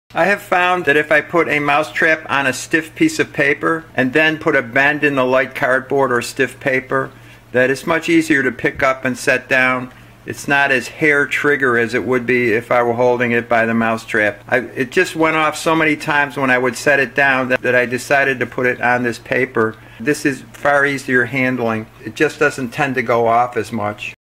Easier Handling for a Mousetrap
Your mouse trap won't tend to go off as easily if you set it on a piece of paper or a light piece of cardboard. If when you are handling the the mouse trap it just tends to go off a lot then that's a dangerous situation you don't want that. It's much safer if you just put it on this piece of stiff paper or an index card perhaps and then it will handle much easier for you and you'll be a happier person and the world will be a better place.